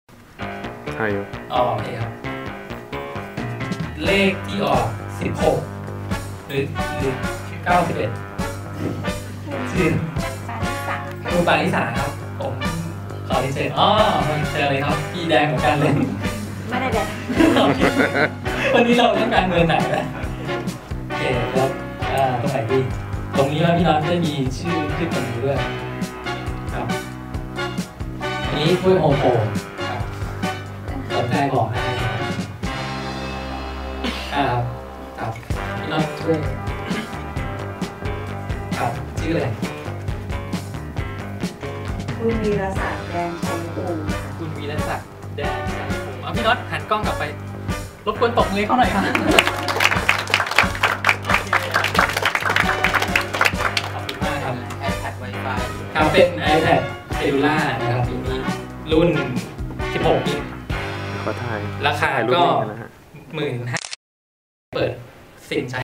ประกาศผู้ได้รับ iPad Mini รุ่น9
ประกาศแล้วผู้ที่ได้รับรางวัลจากการส่งการบ้าน เข้ามาของรุ่นที่ 9 และ จับรางวัลในรุ่นที่ 10